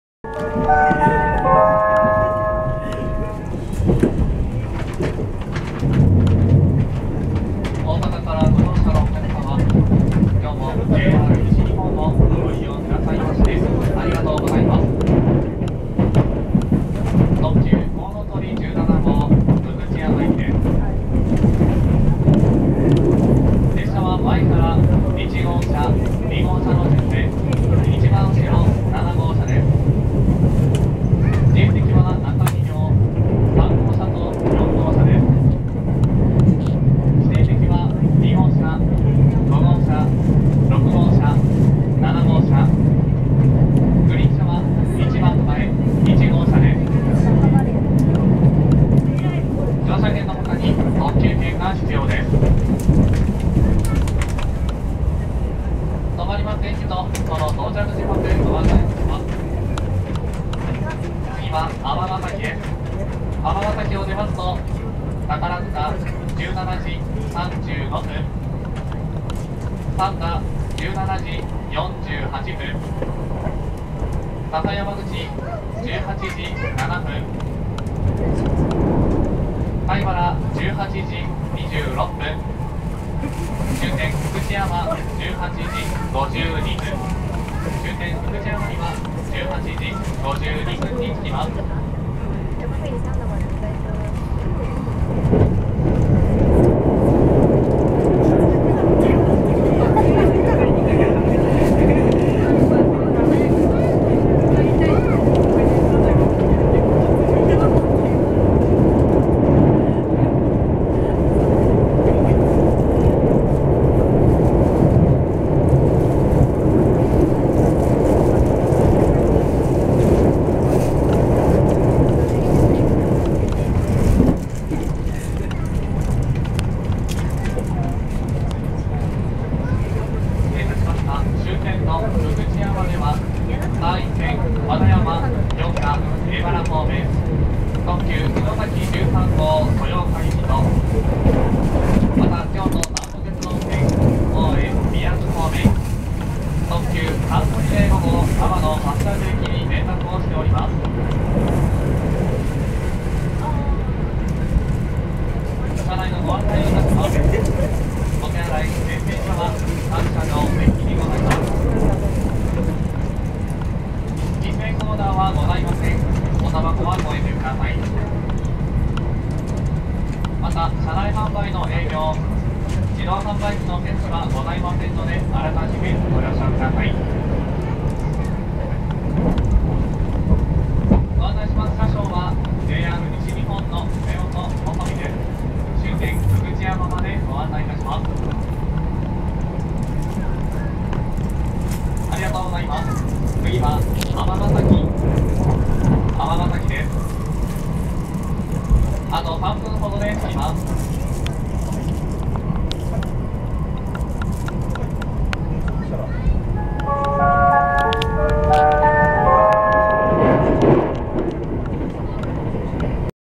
289系特急こうのとり17号福知山行き 大阪駅発車後車内放送
10月31日よりデビューした289系特急こうのとり17号福知山行きの大阪駅発車後車内放送です。15.11.03